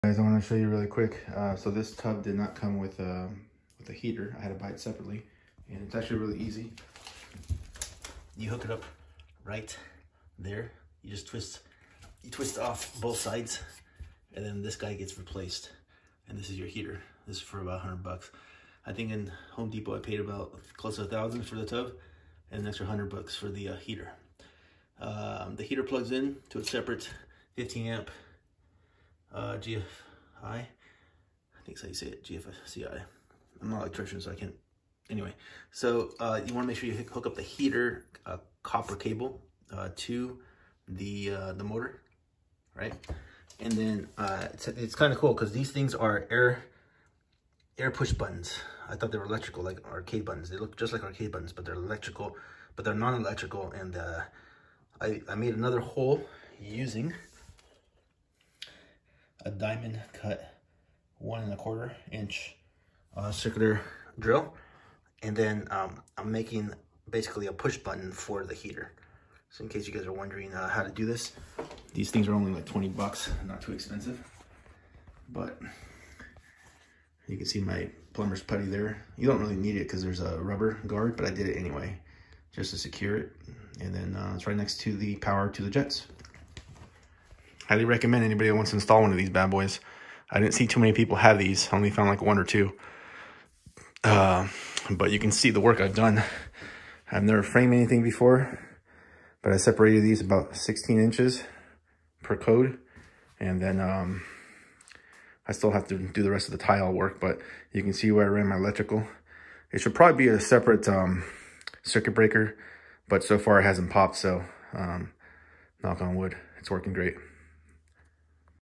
DIY Corner American Standard Whirlpool with Heater Add-on / Mod
Corner American Standard Whirlpool with Heater Add-on / Mod

Used a diamond drill bit to make the shoe through fiber glass.

Fun project highly recommend it for those working on computers all day like me.

Cesar
CesarRamirez.com/